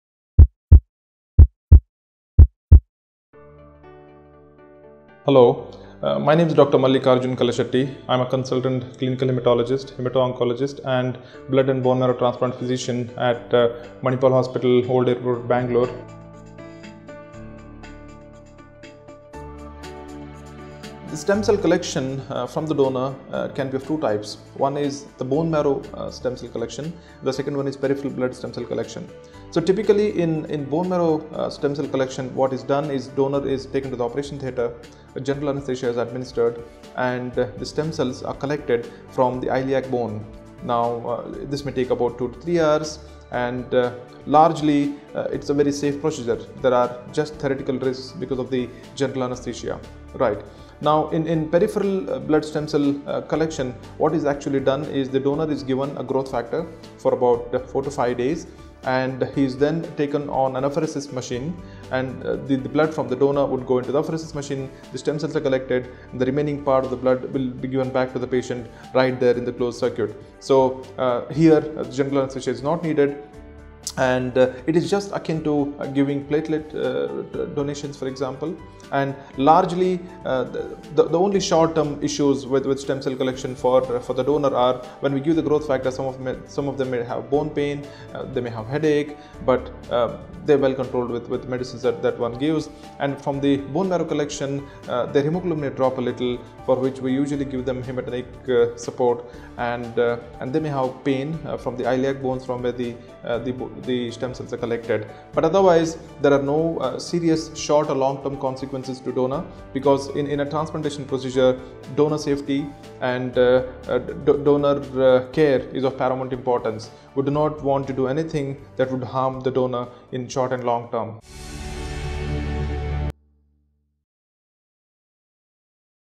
How does one collect stem cells from the donor? - Dr. Mallikarjun Kalashetty
The stem cell collection from the donor can be of two types. One is the bone marrow stem, cell collection. The second one is peripheral stem cell collection. So typically in the bone marrow stem cell collection, what is done is the donor is taken to the operation theater, general anesthesia is administered, the stem cells are collected from the iliac bone. Now, this may take about 2 – 3 hours. Largely, it is a very safe procedure. There are just theorical risks because of the general anesthesia. Now in peripheral blood stem cell collection, what is done is the donor is given a growth factor for about 4 -5 days and he is then taken on an anapheresis machine and the blood from the donor will go into the anapheresis machine  . The stem cells are collected. The remaining part of the blood will be given back to the patient right there in the closed circuit. So here, general anesthesia is not needed and it is just akin to giving platelet donations for example, and largely the only sort term issue with stem cell collection for the donor are when we give growth factor, some of them may have bone pain, they may have headache, but they are well controlled with medicines that one gives and from the bone marrow collection , the hemoglobin may drop a little and  which we usually give them hematric support and then they have pain from,t he leg bones, the stem cells are collected. But otherwise there are no serious short or long term consequences to the donor, because in a transplantation procedure, donor safety and donor care is of paramount importance. Would not want you to do anything that would harm the donor in a long or short term.